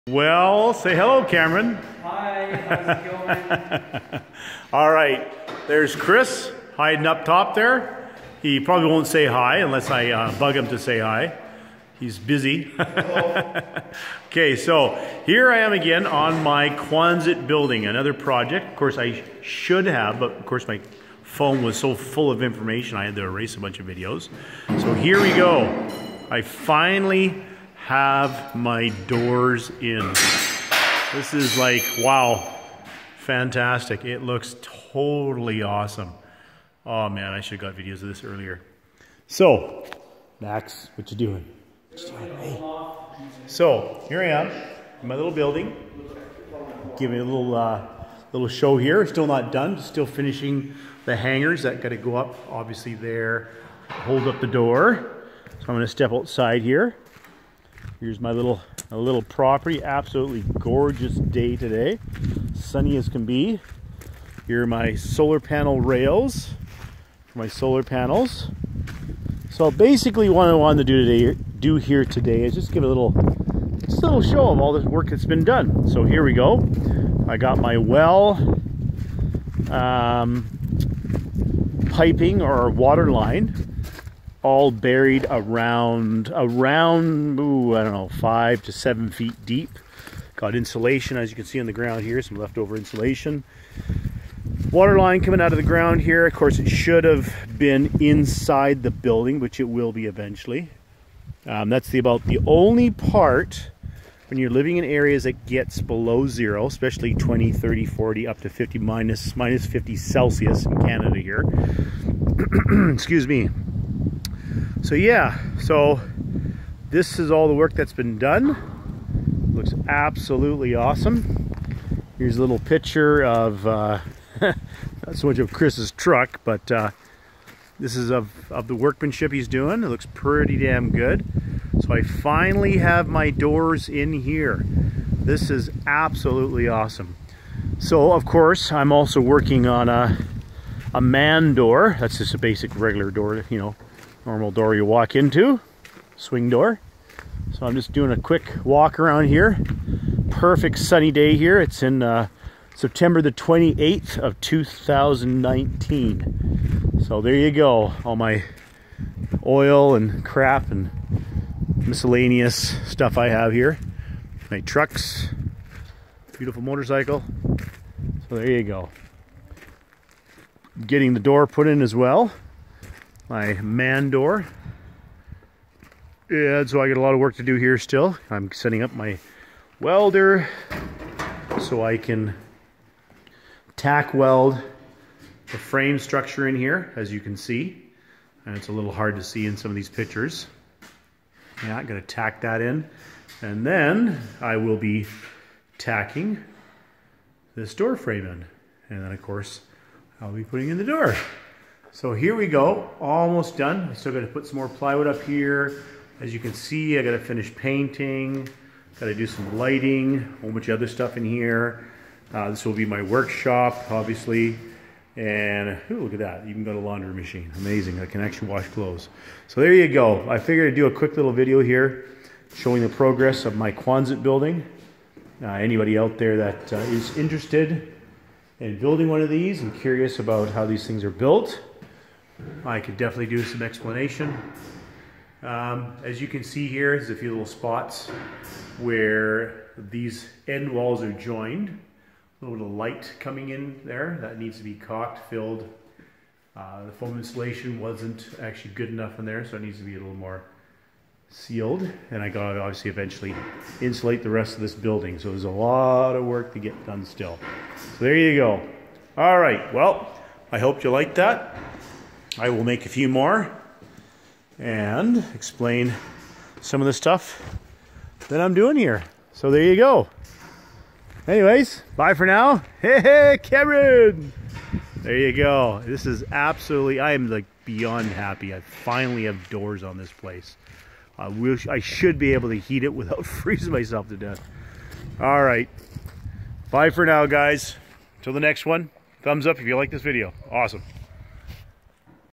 Installing garage doors on my quonset building with my bros. Cameron and Chris
It was a lot of fun and a great sense of accomplishment to finally get the big garage doors in.
 Thanks to my friends Chris & Cam.